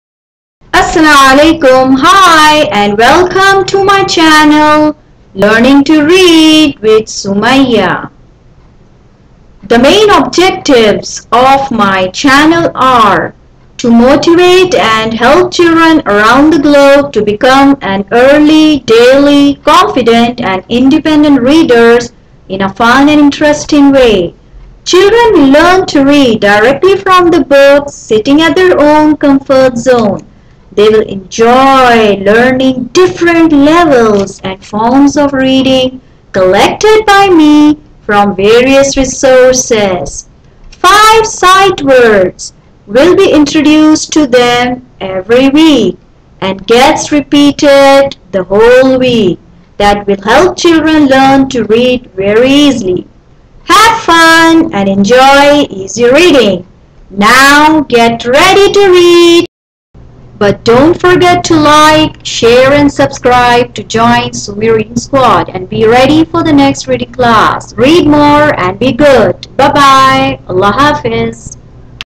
Learning to read with Sumyyah!!- Introduction
The main purpose of my channel is to help, motivate children and also adults who struggle to read around the world. As in most of the countries, children are deprived of good reading books and libraries, so through my channel they will be able to read and see different levels of books which will help them to develop their reading skills.